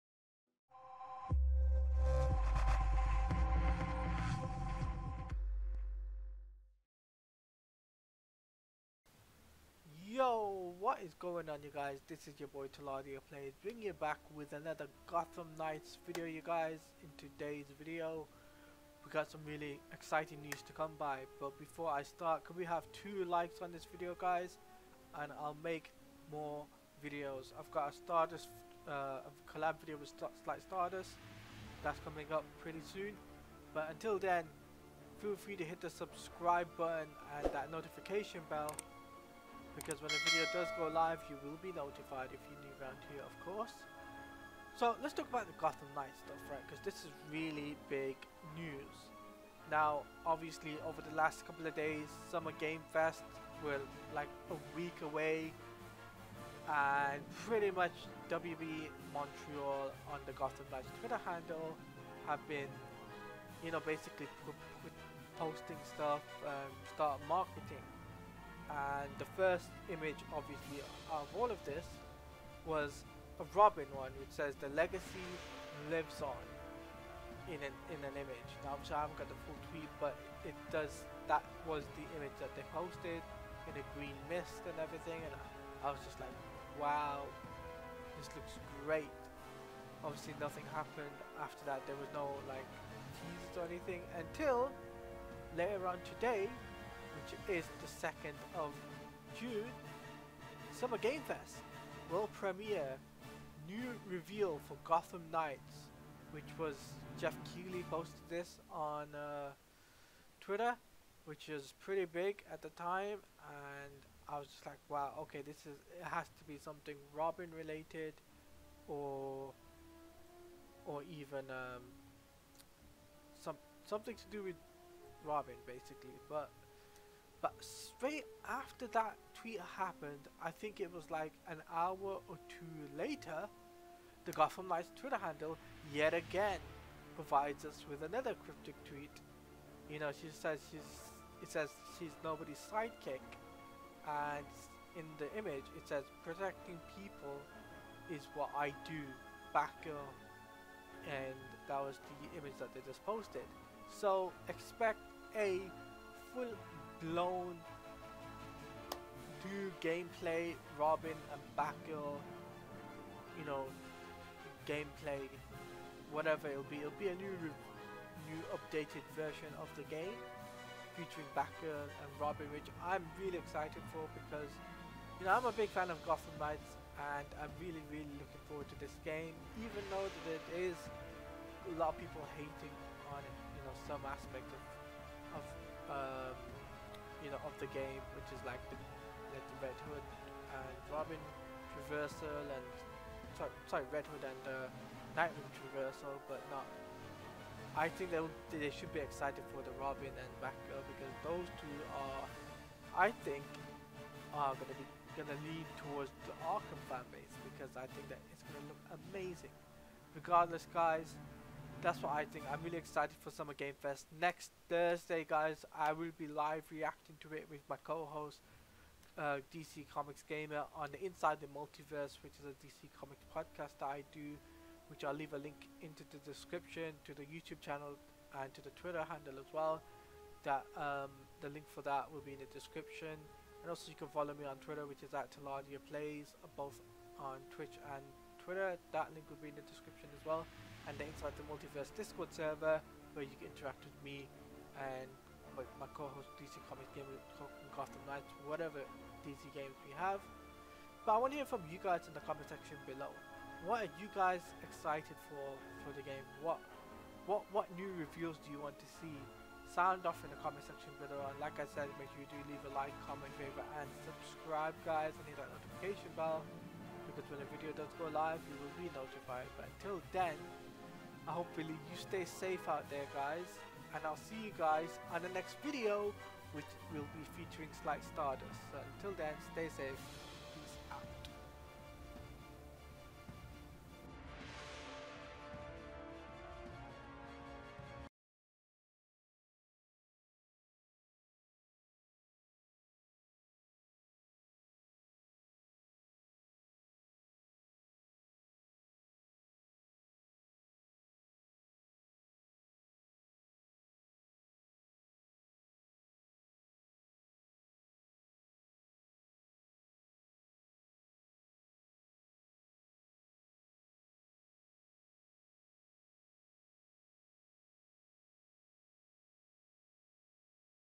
Gotham Knights NEW REVEAL AT Summer Games Fest Confirmed!
In today's Video i talk about the new news that we are getting New reveals at Summer games fest next week which may feature robin and Batgirl
Leave Your thoughts on the new news in the comment section below!